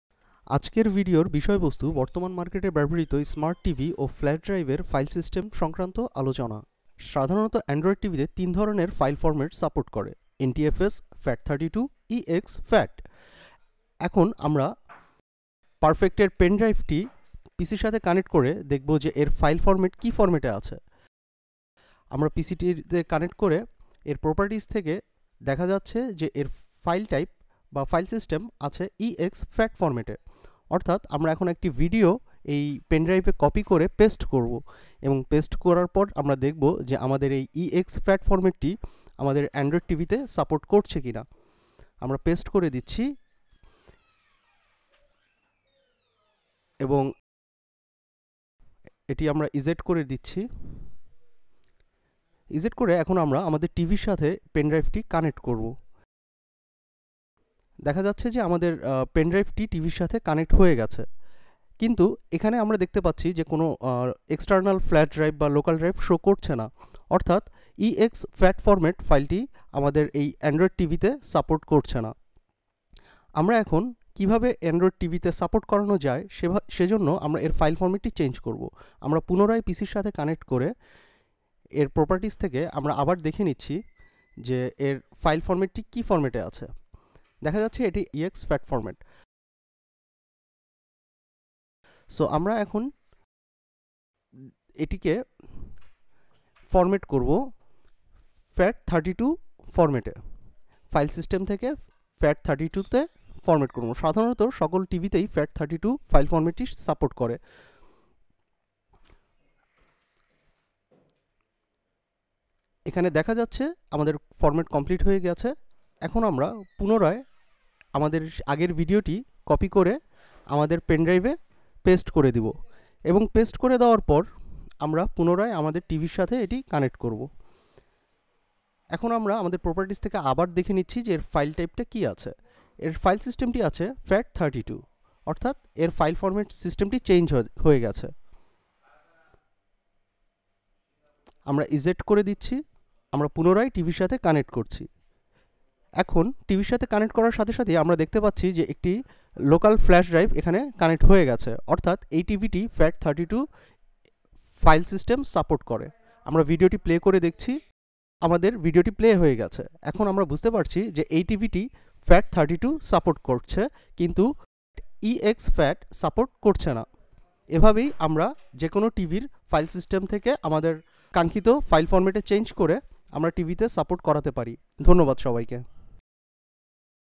Flash Drive & Smart TV File System Compatibility
How to compatible File System (FAT32/exFAT/NTFS) with Flash Drive & Smart TV